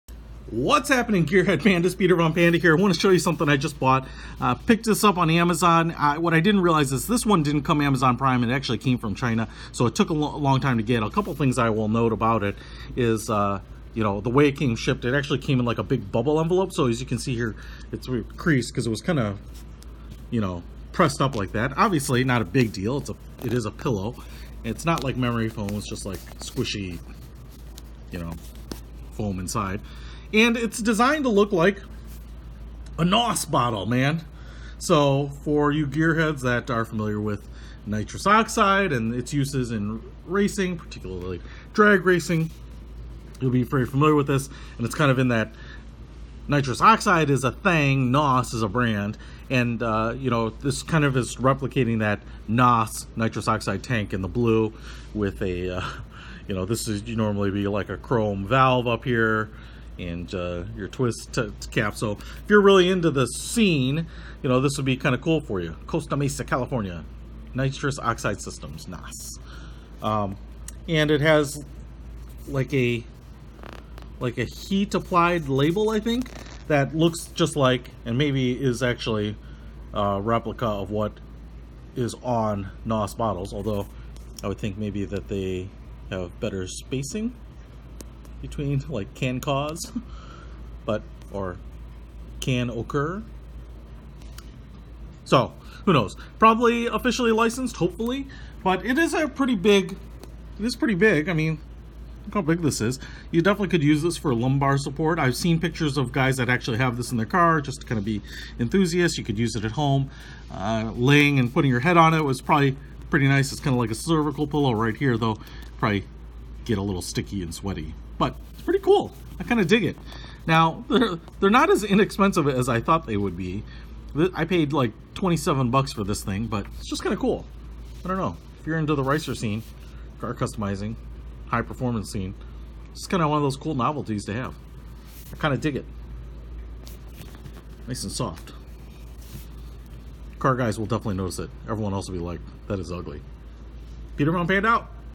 NOS Nitrous Oxide Pillow Cushion Review
NOS
NOS Travel Pillow,Jacksuper Super Cool Memory Foam Car Decor Head Back Rest Sofa Cushion Toy Gift (Headrest(30cm))

Nitrous oxide bottle shape pillow, very creative and funny
Plush material provides soft and comfortable touch feeling
The shape of the long bottle save space, more suitable for car travel
Make you have a good rest
Size: 48cm x 15cm/18.90" x 5.91" (Approx.)

Specifications:
Type: Pillow
Material: Plush
Features: Nitrous Oxide Bottle Shape, Car Decoration, Comfortable, Soft
Notes:
Due to the light and screen setting difference, the item's color may be slightly different from the pictures.
Please allow slight dimension difference due to different manual measurement.
Package Includes:
1 x Pillow Expedition Money LLC
A Financial Wellness Program That Works